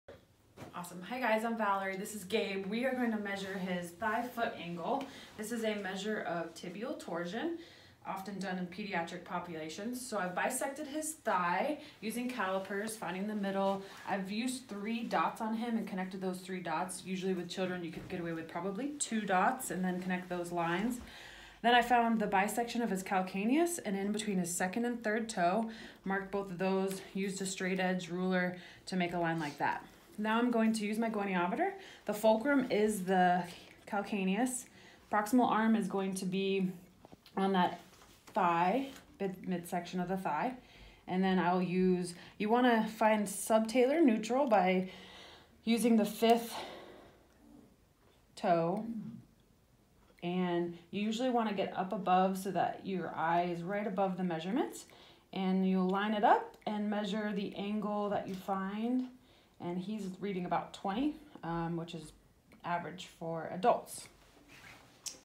Thigh foot angle
Measure amount of tibial torsion (do not use if foot abnormalities present). Normal: ~10 degrees.